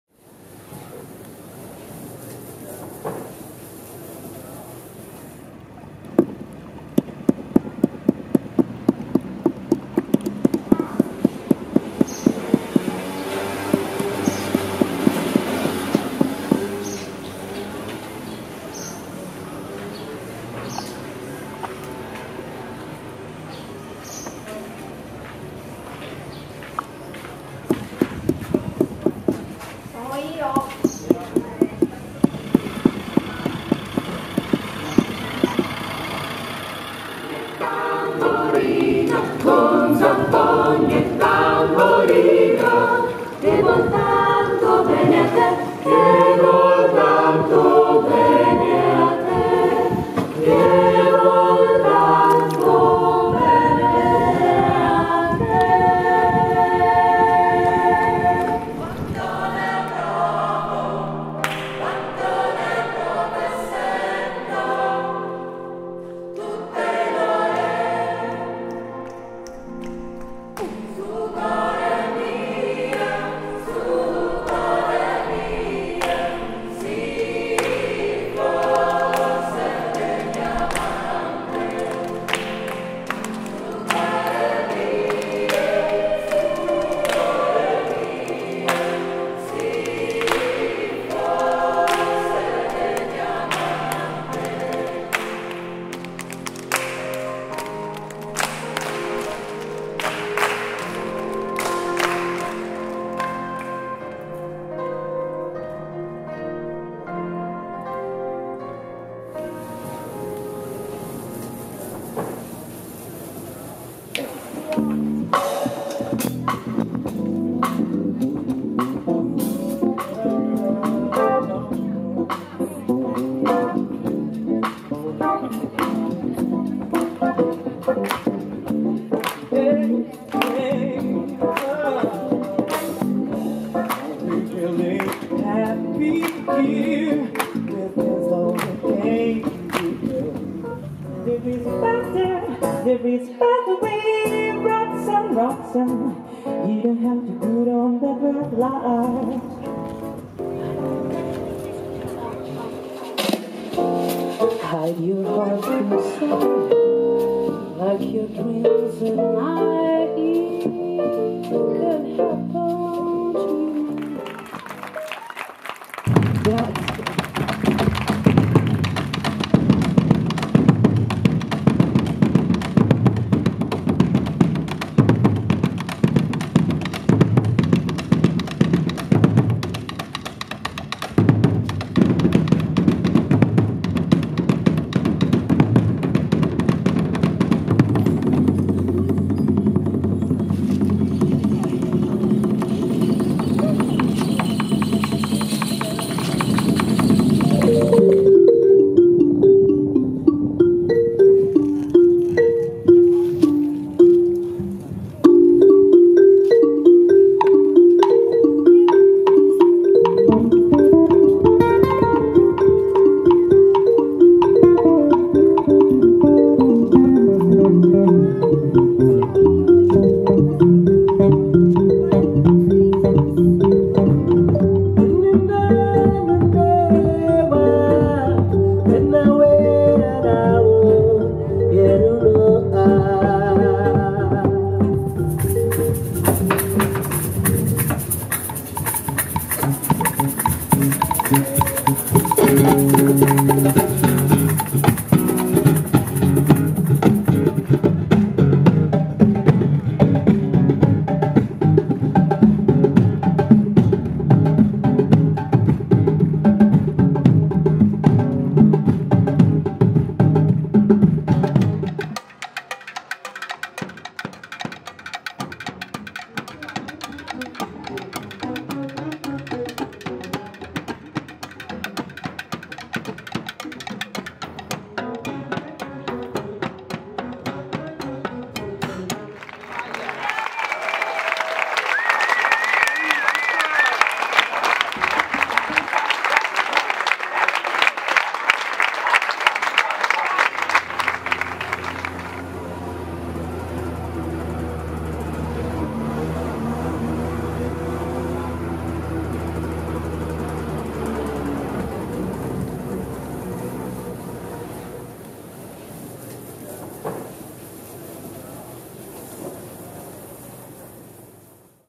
RUMORI NELL'ISOLA 2015
RUMORI NELL'ISOLA- Jazz Festival in Ventotene Island (Italy), 3-4-5 Settembre 2015 //Artists: -Maria Pia de Vito & Vocal Geographies -Ruggero Artale Afro percussion quartet //Festival Art Director: Angelo Romano Multimedia by Sandunga© (Manuel Garcia, Alessio Castagna)